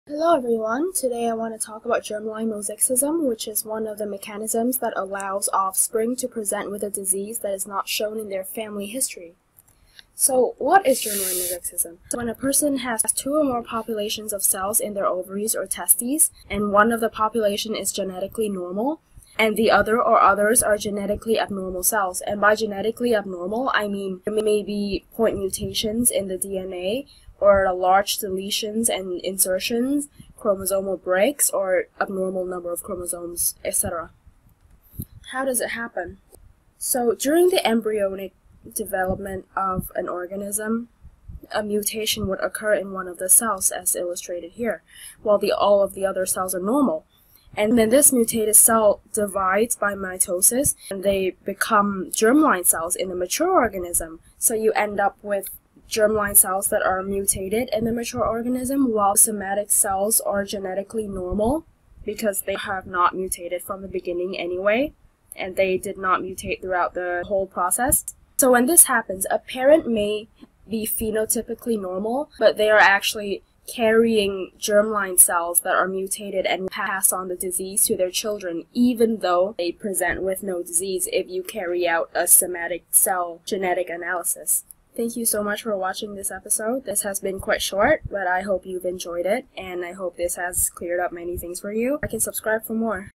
Mosaicisms - Part 1 - Germline Mosaicism
-Josephine. :D
Germline mosaicism - what it is, the mechanisms by which it works, and its effects.

Credits to Julia for the diagrams.